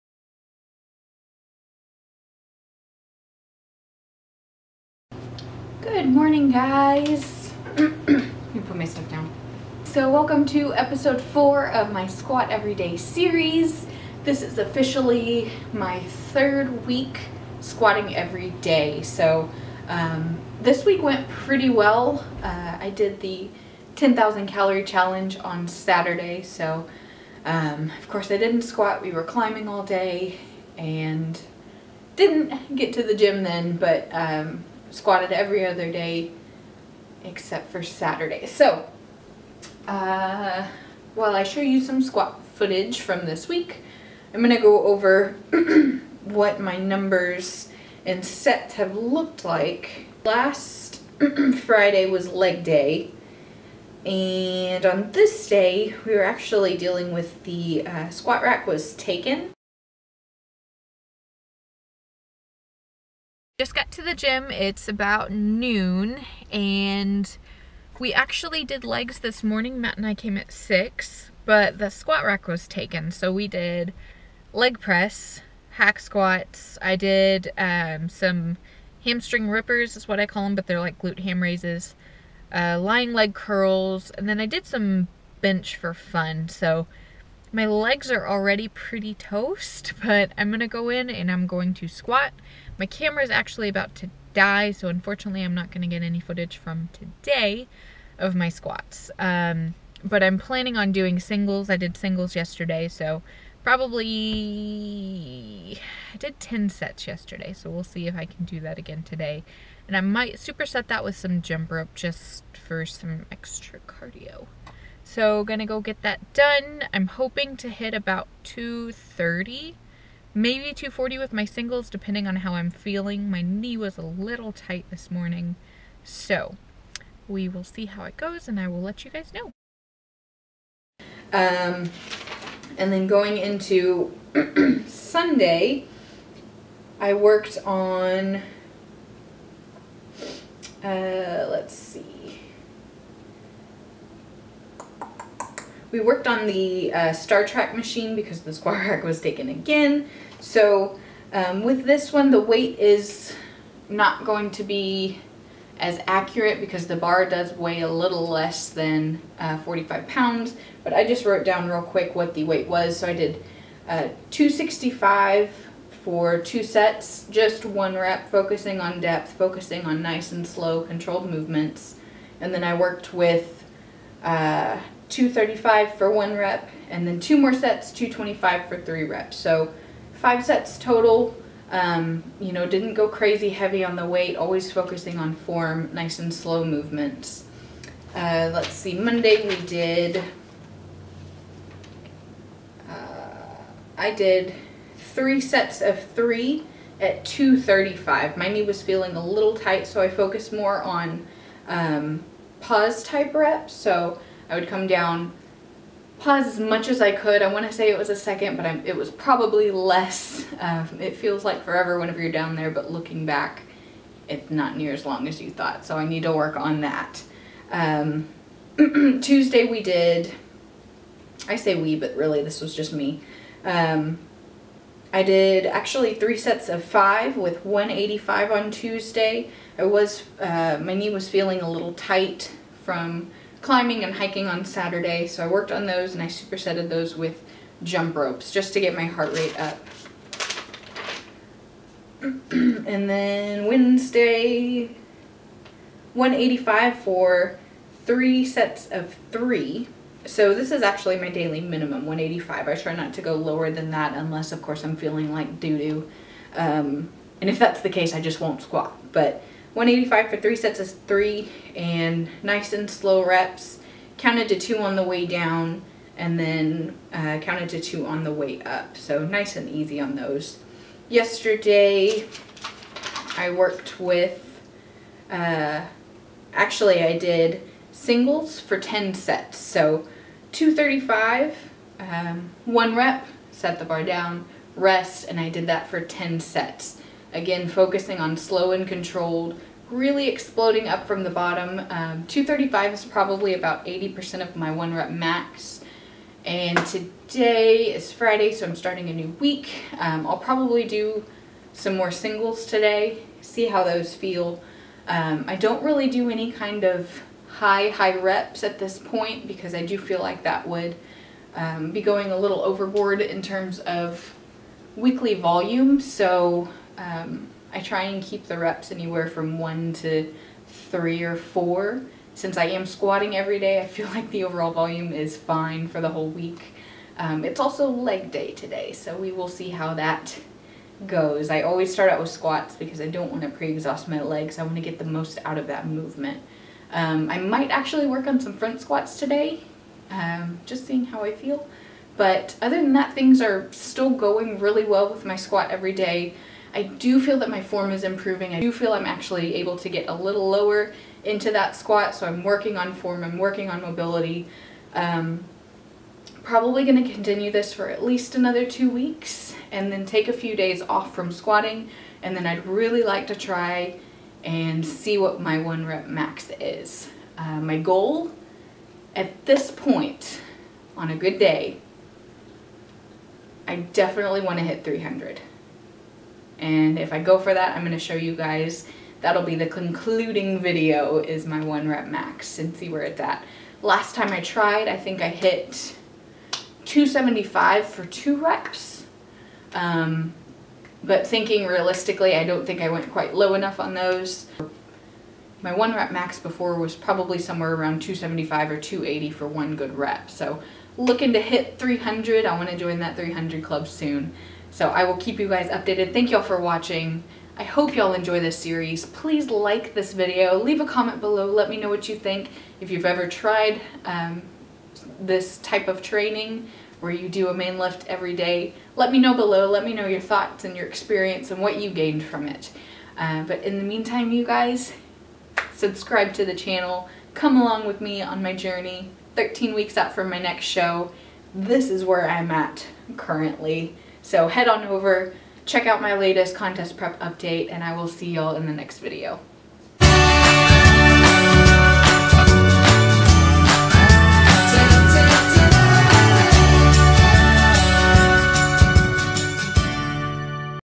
SQUAT EVERY DAY - SED SERIES - EPISODE 4: 1RM GOAL
I'm done with my full third week of squatting every day!!! Since starting I have noticed a good improvement in my overall mobility and I feel that I don't struggle to warm up as much as I did when I wasn't squatting so frequently...take a look to see more updates from this week, as well as see what my goals are when it comes to my one rep max!!!

Watch this week's update and LIKE UP THIS VIDEO!